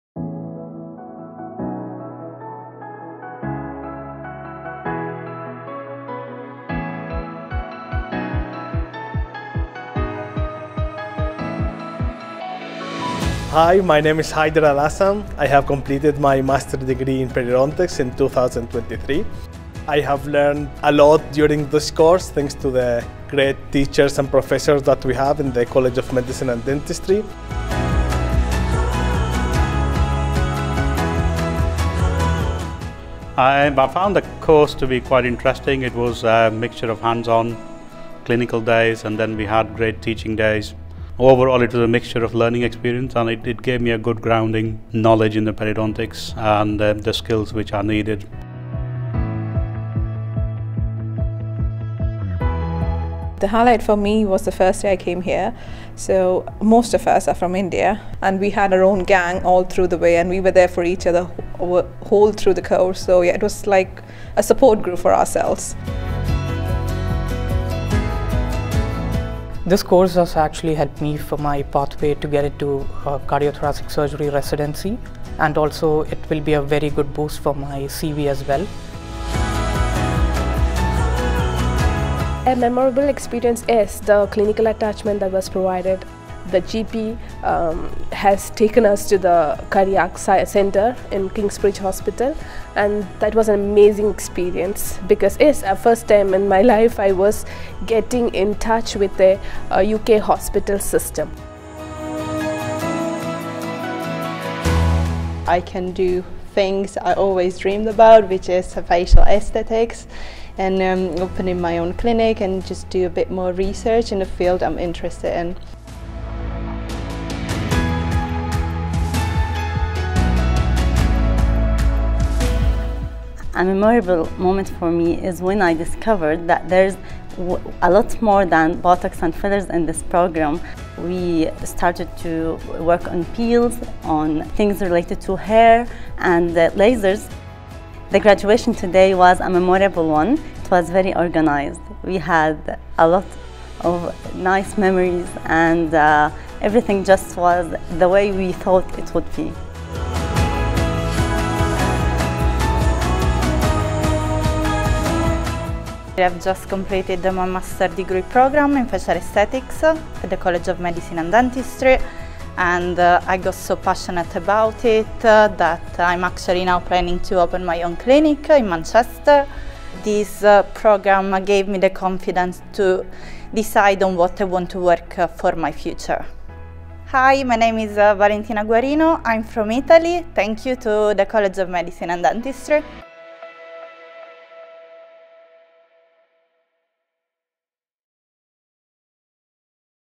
MSc Graduates Share Stories of Excellence at College of Medicine and Dentistry
Get inspired by the heartfelt journeys of our students in the MSc programmes at College of Medicine and Dentistry - Ulster University. 🎓🌟

Listen to their stories, and envision your own path to excellence.

Join us in celebrating success and the pursuit of knowledge, because at CoMD, every journey matters.